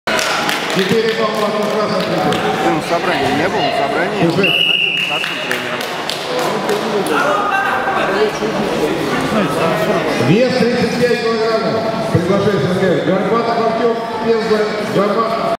ОБЛАСТЬ-ЧУМЕРОВ 2014 92
Турнир памяти Владимира Чумерова, Область по юниорам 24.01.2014. Греко-римская борьба в Самаре. Greco-Roman Wrestling in Samara Russia.